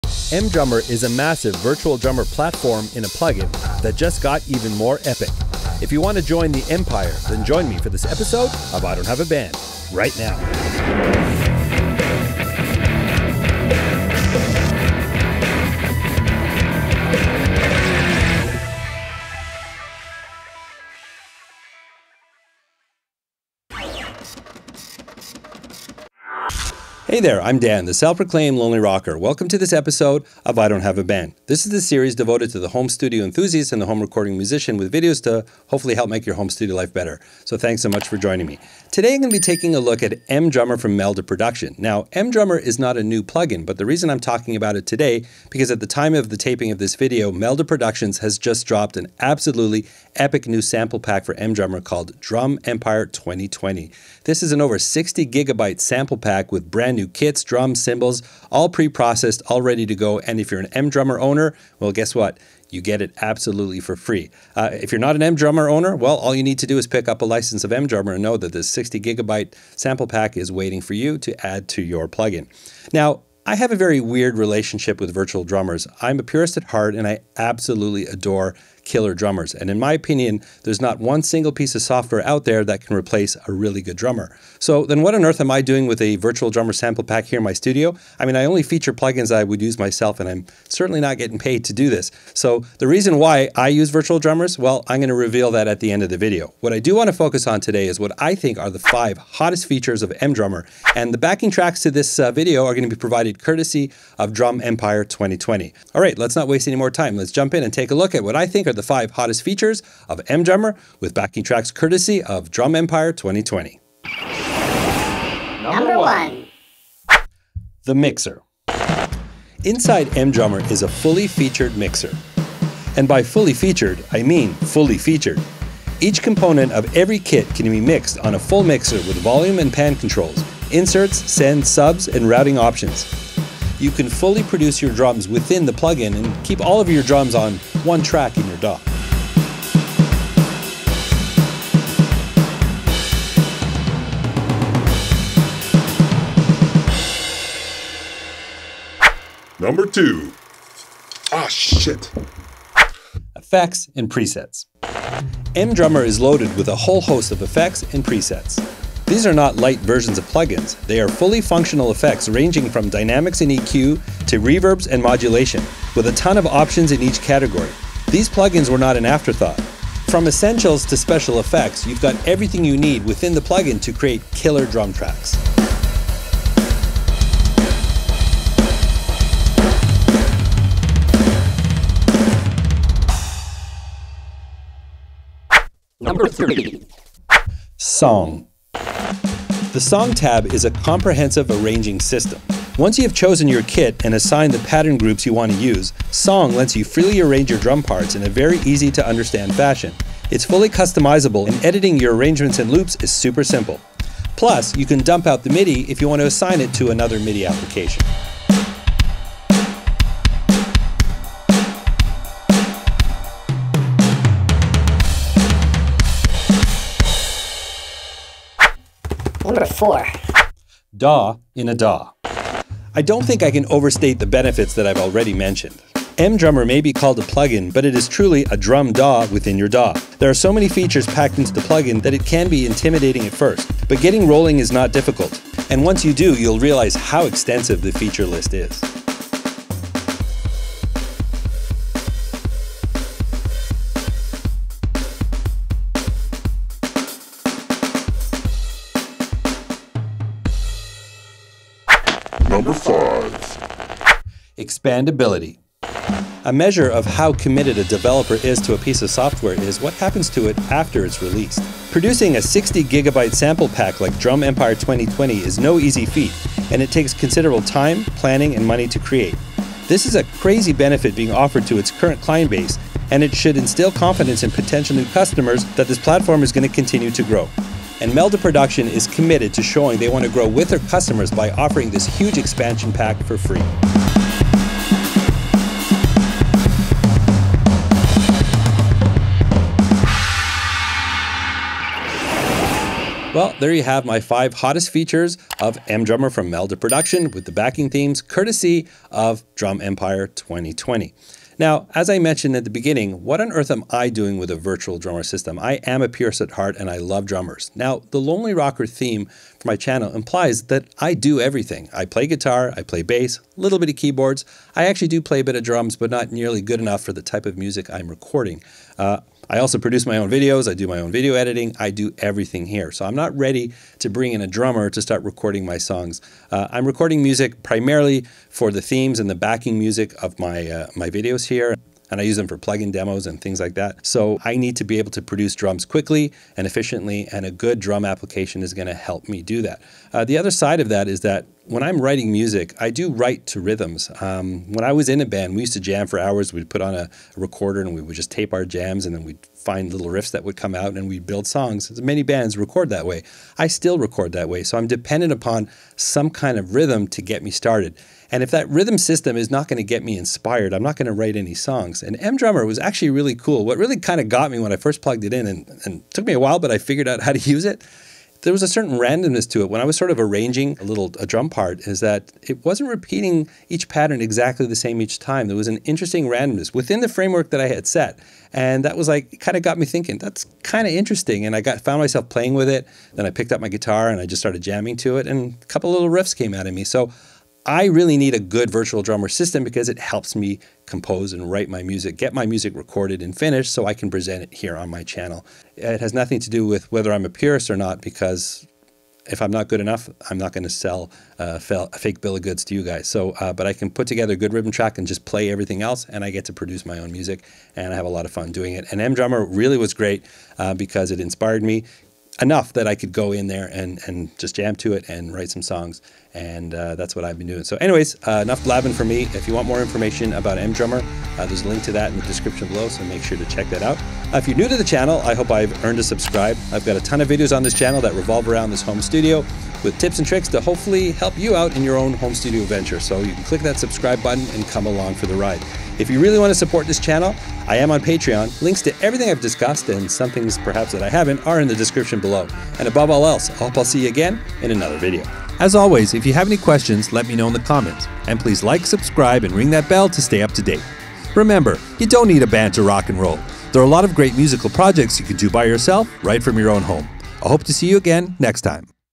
MDrummer | Drum Empire 2020 | Meldaproduction Plugins Review
In my latest Meldaproduction Plugins Review I look at MDrummer and their latest 60gb sample pack, Drum Empire 2020.  This is one of the most complete virtual drummer plugins on the market.

From the developer:
MDrummer is simply the ultimate drum machine and virtual drummer. It makes arranging drum tracks and real-time performances quick and simple and the possibilities are just endless.

MDrummer's sound engine can cover the entire range of percussion instruments from drums to scratches. Any number of drum sets, drums in each drum set, layers and effects in each drum, with sampling, synthesis and some completely unique generators. Whether you are interested in acoustic drums, electric drums, percussion or effects, or any kind of combination, MDrummer can do it.